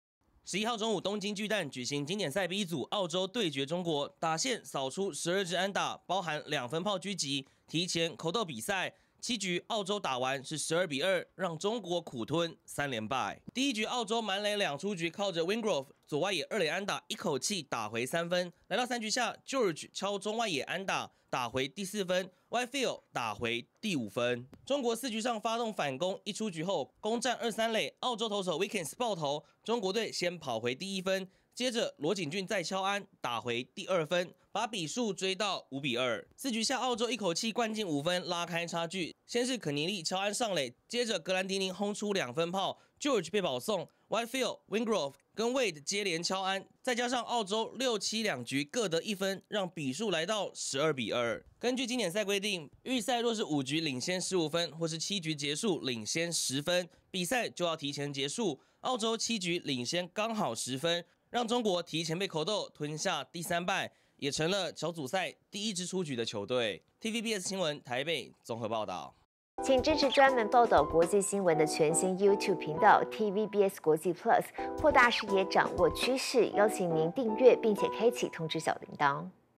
澳洲12:2七局「扣倒」比賽 中國三連敗淘汰｜TVBS新聞@TVBSNEWS01
經典賽B組澳洲對決中國，澳洲打線狂打12安打，還有格蘭迪寧（Robbie Glendinning）2分砲狙擊，澳洲12比2打贏中國，提前在7局就"扣倒"比賽。